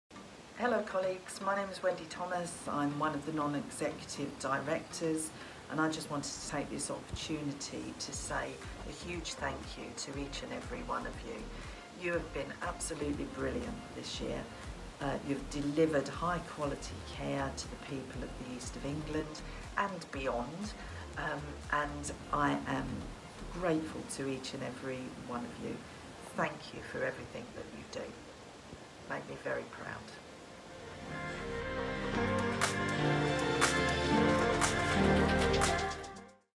Wendy Thomas ThanksTeamEEAST 2021
Wendy Thomas, Non-Executive Director send a message to staff and volunteers at the East of England Ambulance Service NHS Trust (EEAST)